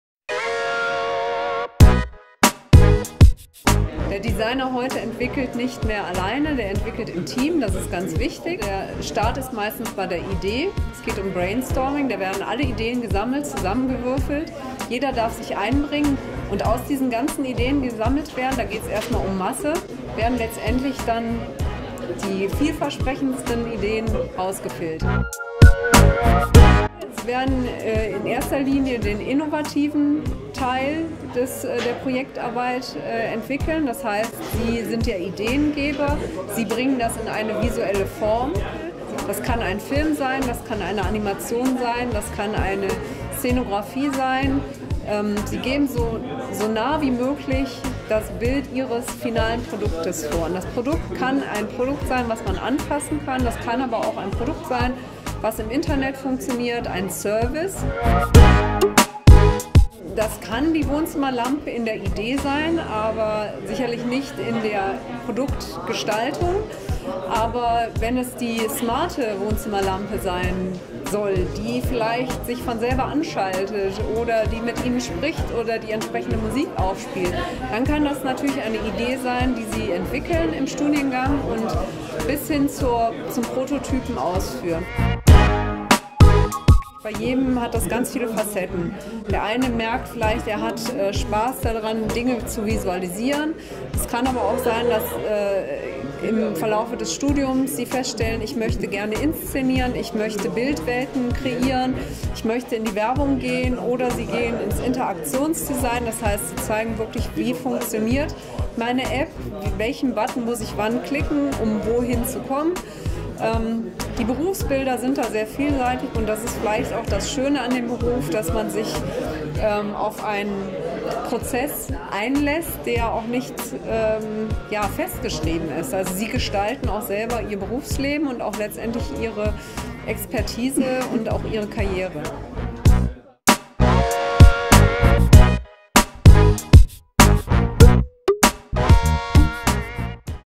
Mediendesign studieren an der Hochschule Macromedia
Welche Kompetenzen vermittelt die Studienrichtung Mediendesign (B.A.)? Für welche Berufe qualifizieren sich die Studierenden, welches sind typische Arbeitgeber?

Das erläutert Professor Ute Hilgers-Yilmaz. Sie unterrichtet Mediendesign am Campus Köln der Hochschule Macromedia.